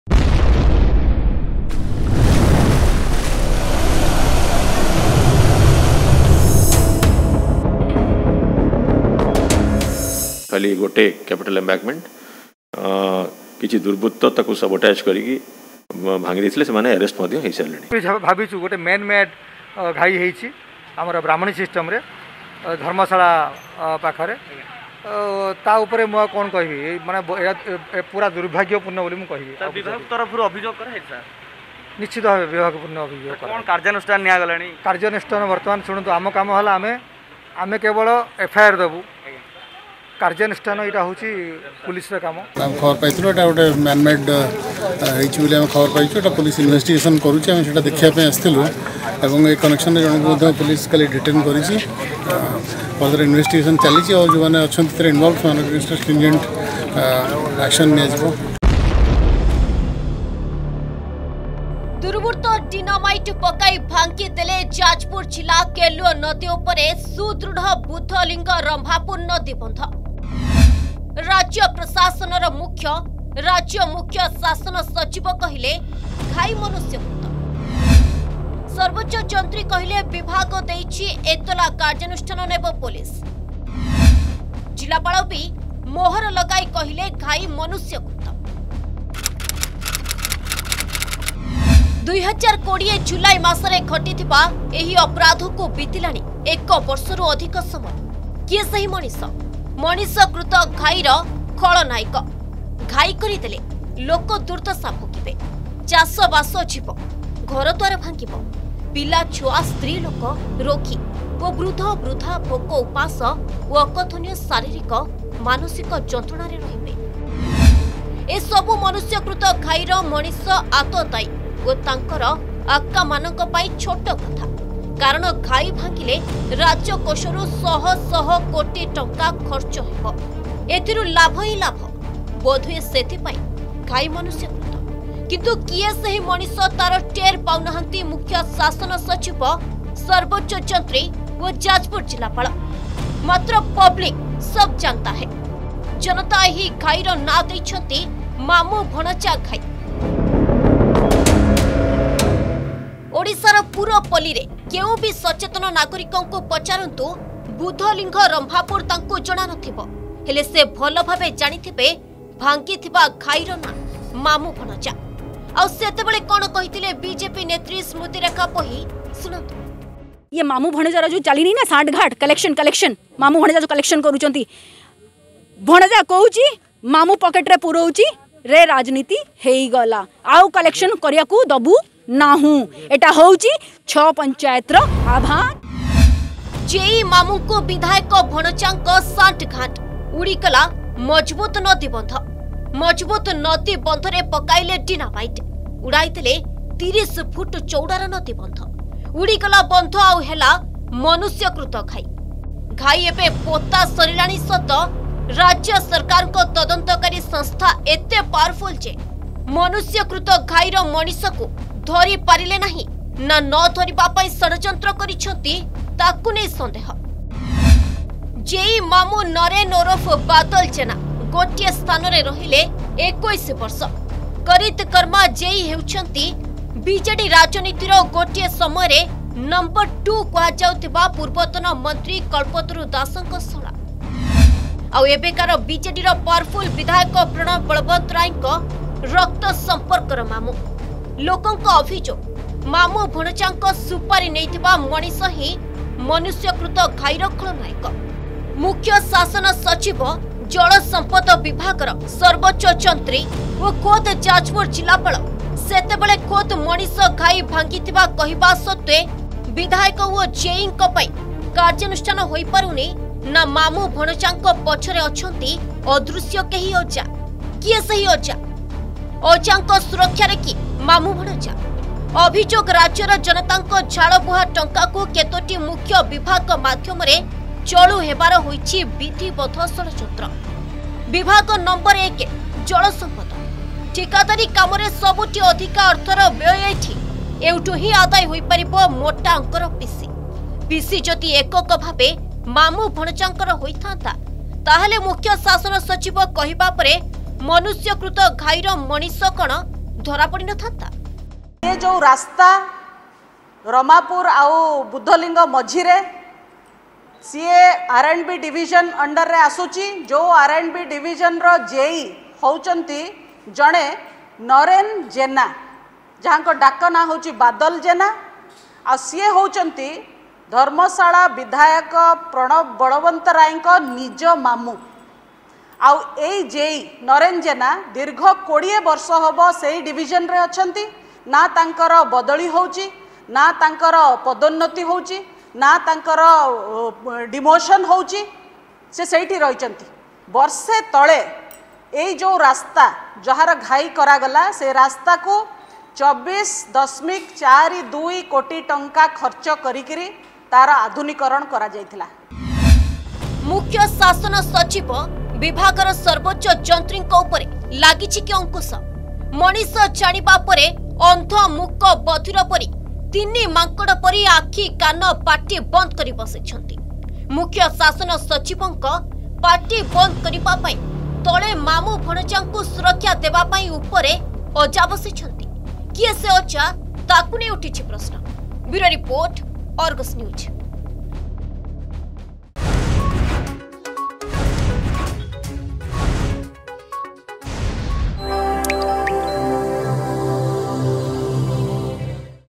ମାମୁଁ ଭଣଜା ଘାଇର କିଏ ସେ ଅଜା ?
ବୁଦ୍ଧଲିଙ୍ଗ-ରମ୍ଭାପୁର ଘାଇକୁ ବତିଲା ଏକ ବର୍ଷ ,ହେଲେ ଧରାପଡିଲେନି ମନୁଷ୍ୟ କୃତ ଘାଇର ମଣିଷ ।
କିଏ ସେ ଖଳନାୟକ ? କିଏ ଦେଉଛି ସୁରକ୍ଷା ।

ArgusNews MamuBhanjaghai Dharmasala Jajpur ChiefSecretary s Odisha , odisha.